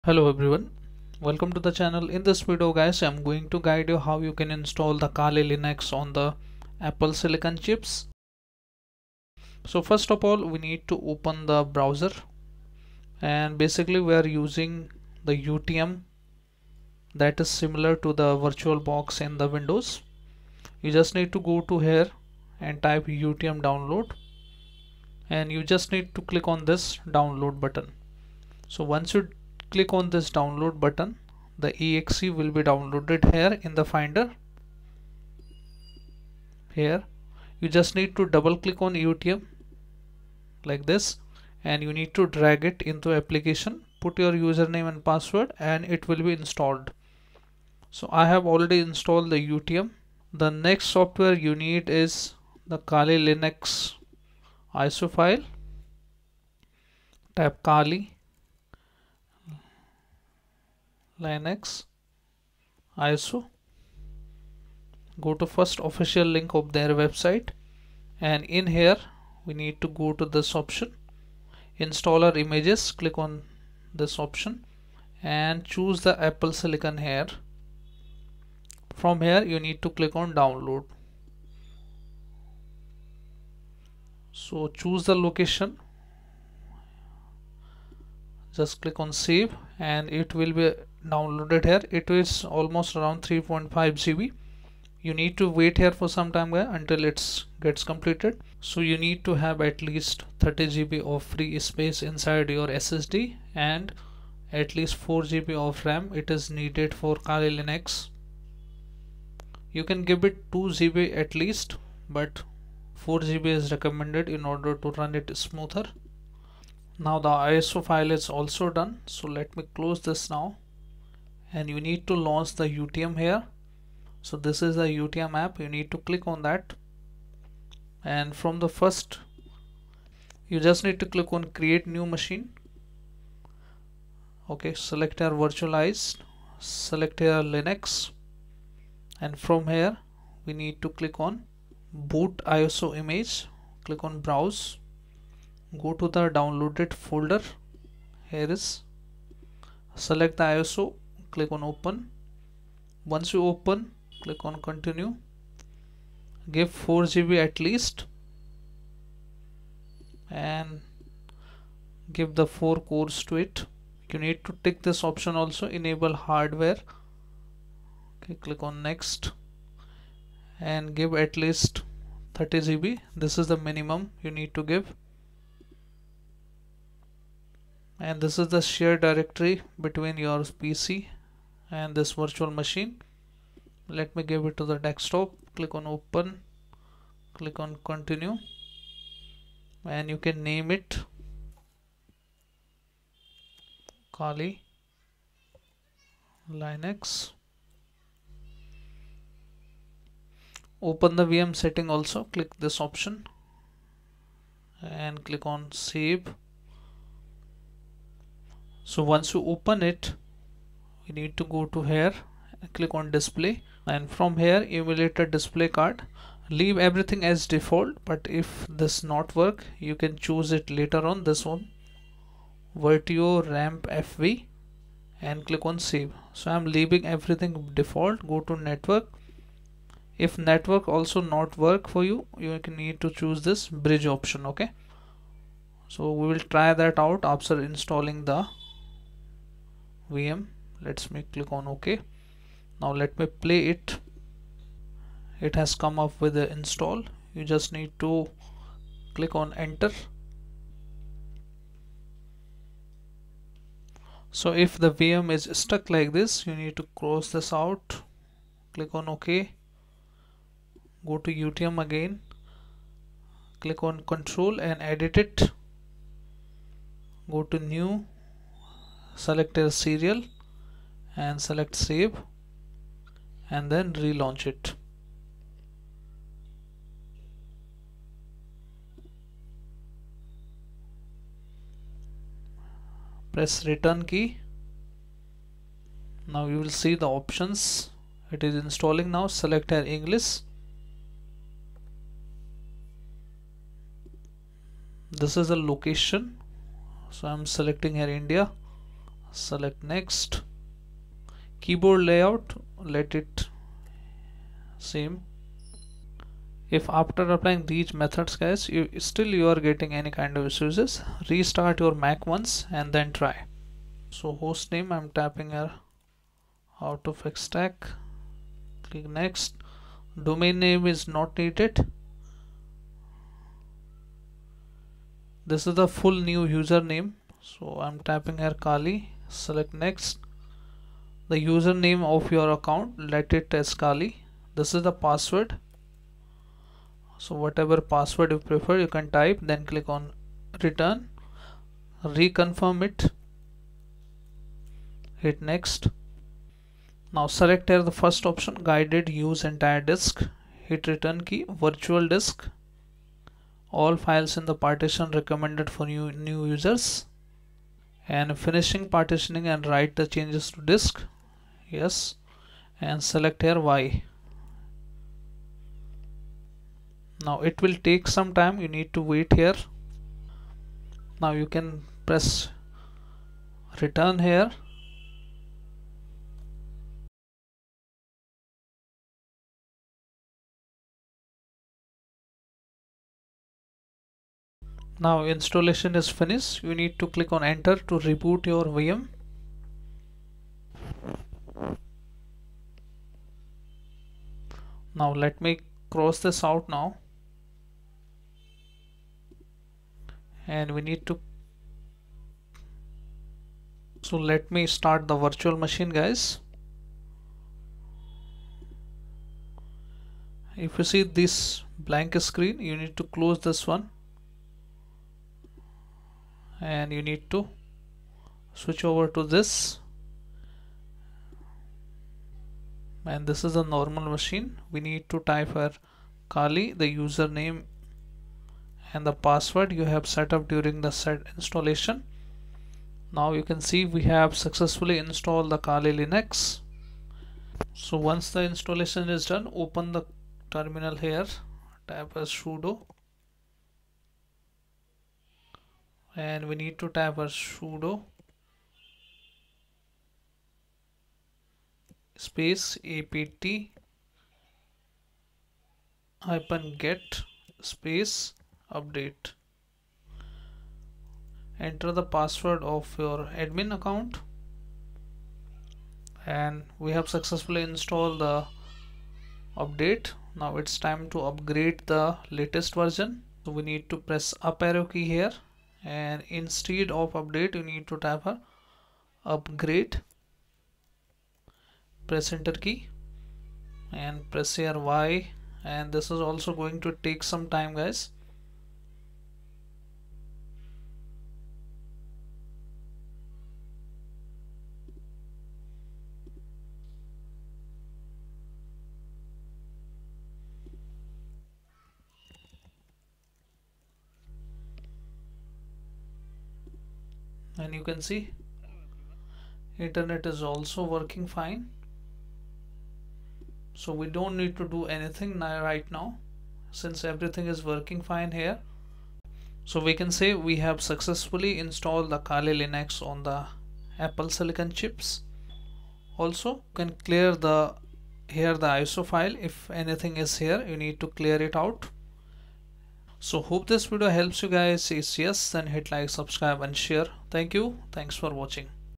How To Install Kali Linux 2024.4 In MAC With Apple Chips (M1 | M2 | M3 | M4 In 2025 Step by Step✅ )
✅ This instructions in this video tutorial can be applied  for  laptops, desktops, computers, which run MacBO . Macbook Air or Macbook Pro operating systems. This tutorial will work on all Cpu hardware manufactures, Intel Macbook, Silicon M1, M2,M4 and M4 .